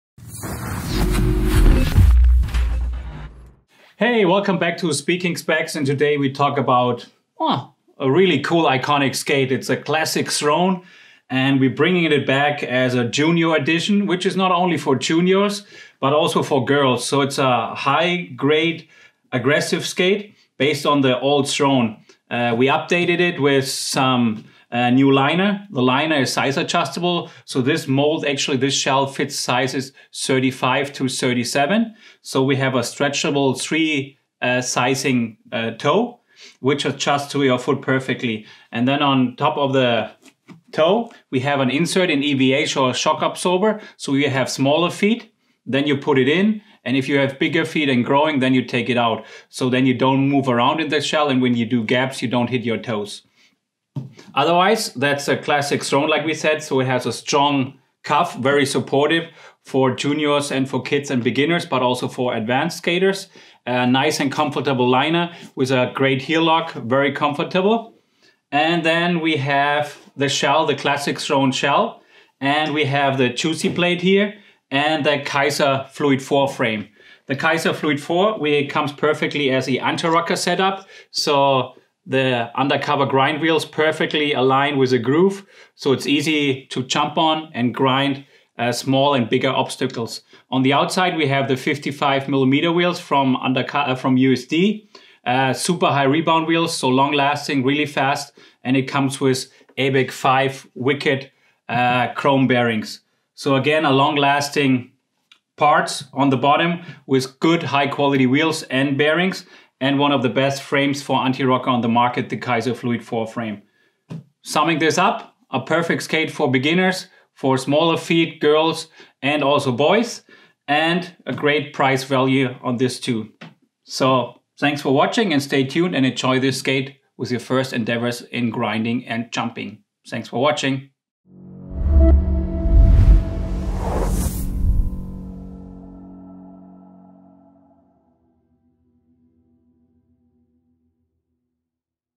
USD Kids Classic Throne - Speaking Specs
USD Kids Classic Throne - Speaking Specs - 700349
Throne Kids
The return of one of the most iconic aggressive skates ever. The Throne Kids is the perfect skate for all future stars on rails and park rats. The shell and cuff are made out of high grade TPU material for support and flex. The soul plate is the famous Juicy plate with a perfect locking groove built in. On the bottom of this UFS skate it comes with a KIZER Fluid IV frame and an anti rocker setup made out of fast and long lasting 55mm wheels with Undercover grindstones in the middle for easy and ultimate sliding experience. Replaceable cuff and top buckle, as well as soul plate make this skate the ultimate dream for any street warrior and is also perfect for girls and women with smaller feet. One size fits all from 35 to 37 with the stretchable liner from USD, the first of its kind and creator of the new dual sizing revolution in skating.